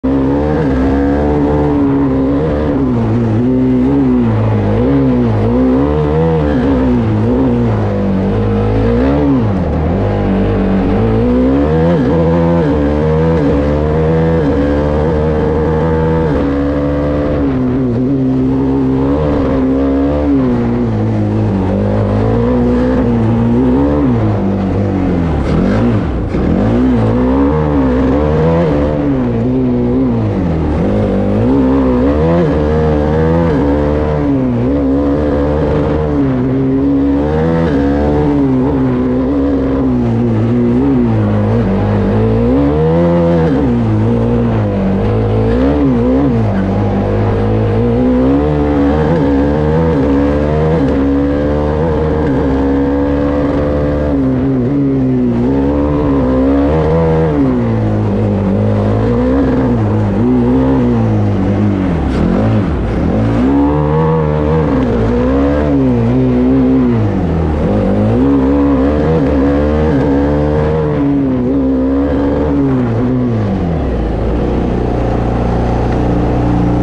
Formula Student Drivers Eye Onboard - Wet Testing - High Speed Karlsruhe - 2021
Two wet laps from a test day back in 2021